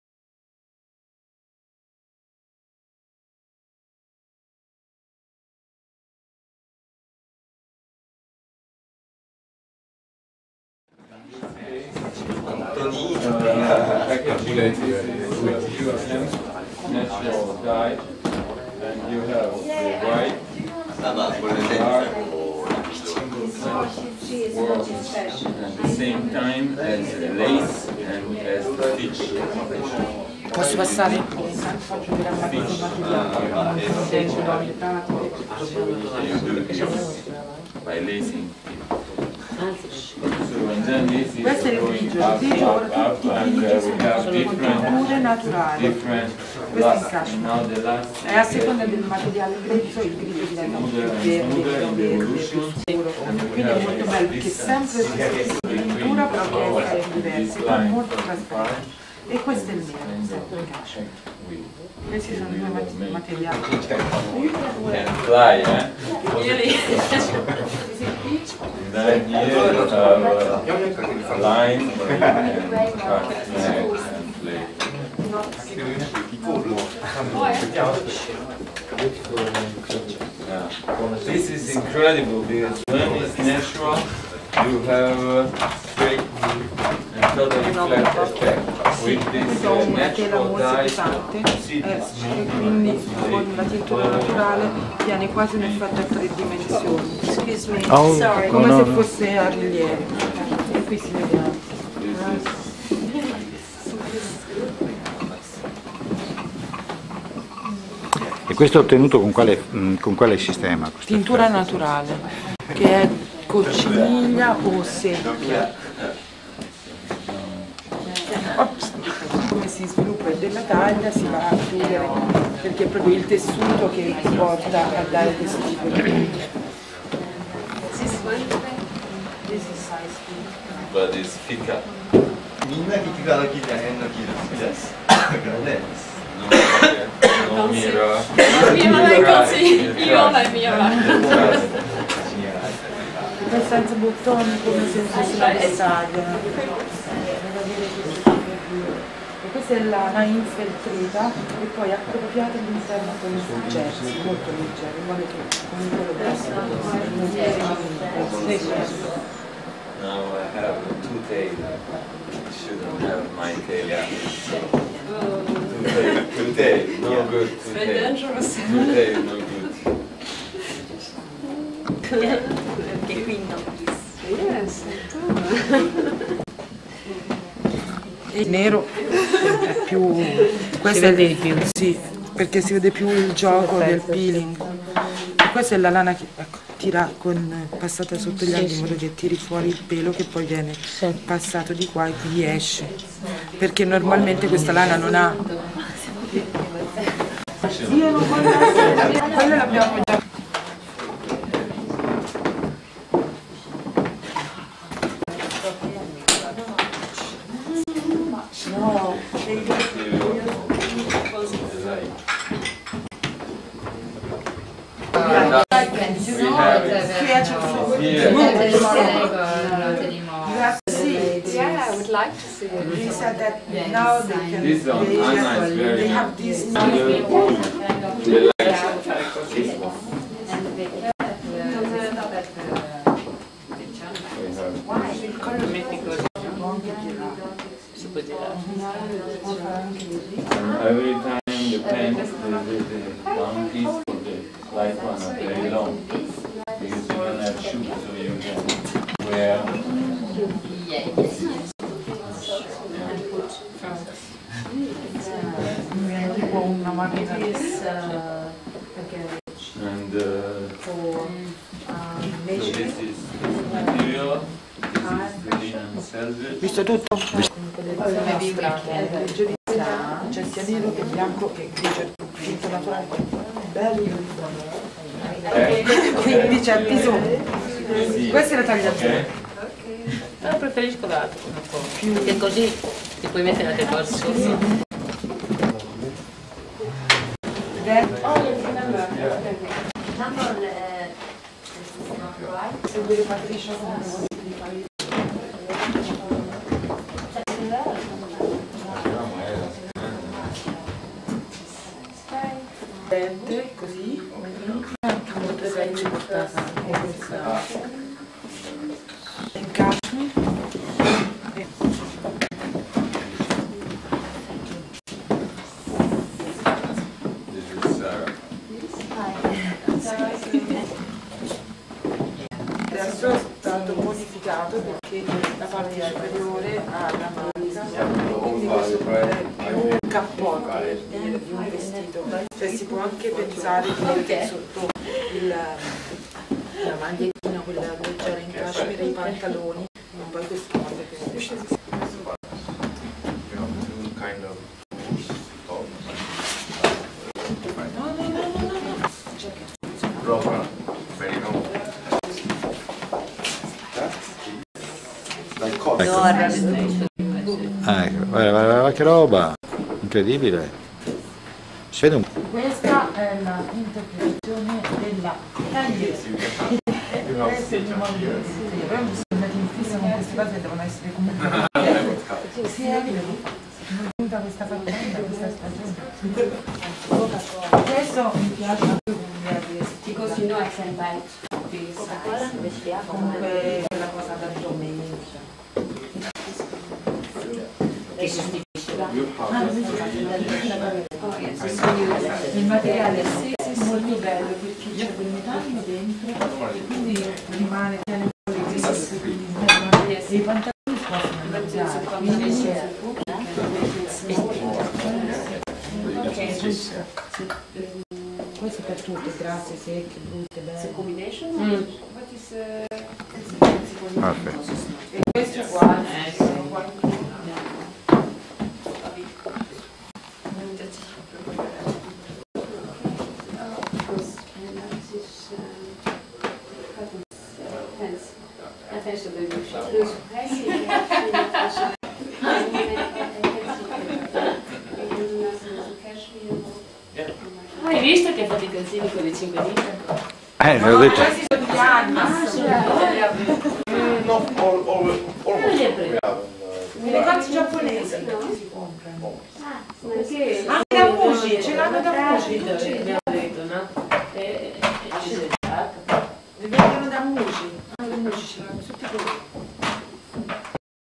The choice of IVO MILAN: Carpe Diem Showroom Autumn/Winter 2003-04
At work with IVO MILAN at Carpe Diem showroom in Paris.

IVO MILAN Archive